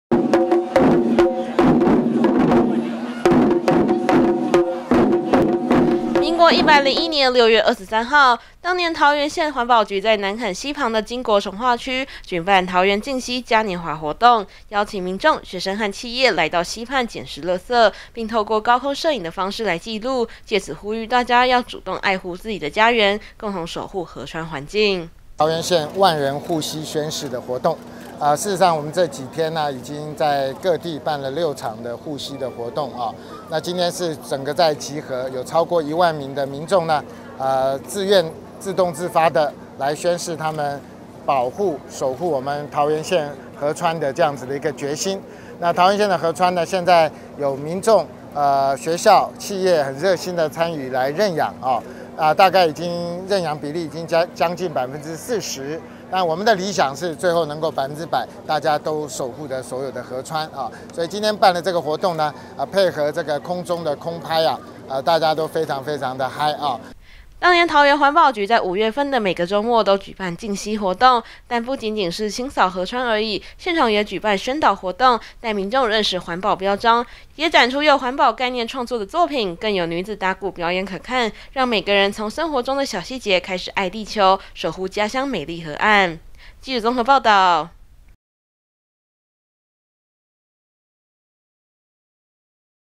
歷史 回顧|桃園淨溪嘉年華 守護河川樂活愛地球
歷史回顧當年桃園南崁溪旁舉辦的淨溪嘉年華

民國101年6月23日，當年桃園縣環保局在南崁溪旁的經國重劃區，舉辦桃園淨溪嘉年華活動，邀請民眾到溪畔來撿拾垃圾，並透過高空攝影來記錄，共同守護河川環境，請看今天歷史上的桃園。